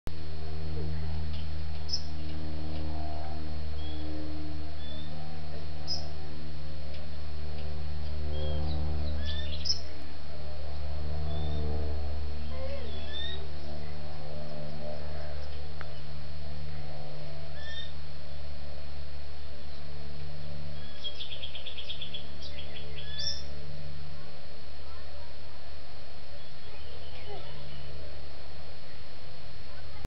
Darshan Trampolines
Darshan, age 13 months, explores the trampoline